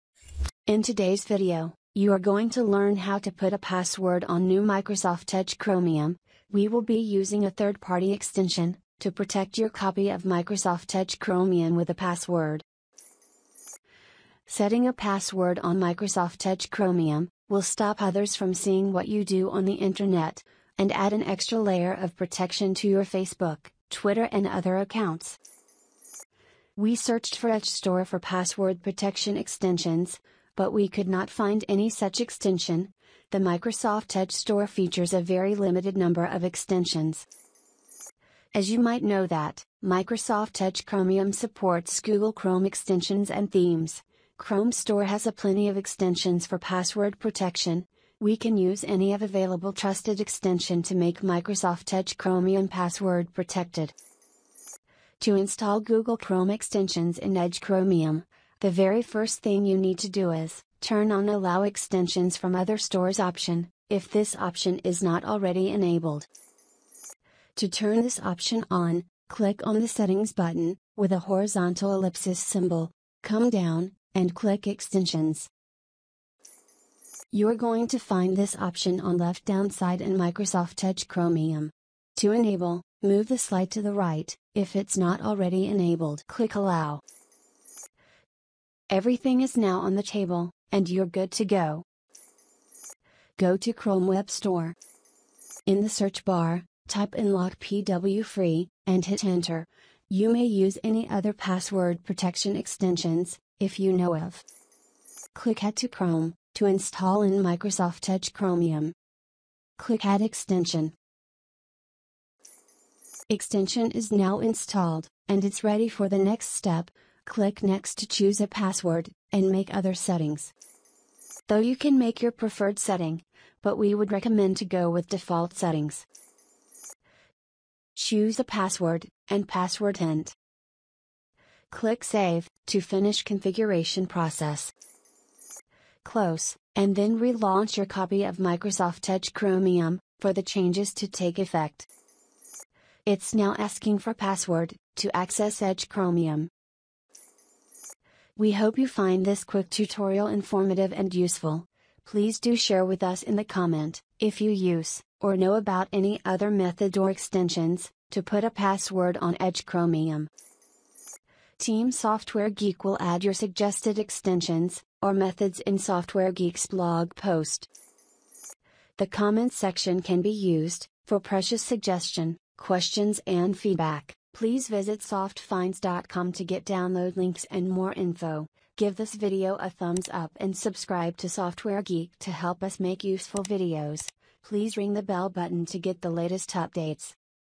How To Password Protect Edge Chromium with A Chrome Password Extension To Make Edge Chromium
Password protect Microsoft Edge Chromium with a Microsoft Edge Chromium password extension for free. This extension can be installed Chrome Web Store free of cost.

This tutorial on making Microsoft Edge Chromium browserpassword protected is quick yet easy to follow.

How to Set a Password on Google Chrome | How to Lock Google Chrome Browser With a Password

We hope You find this quick tutorial informative and useful. Please do share with us in the comment If you use or know about any other method or extensions to put a password on Edge Chromium.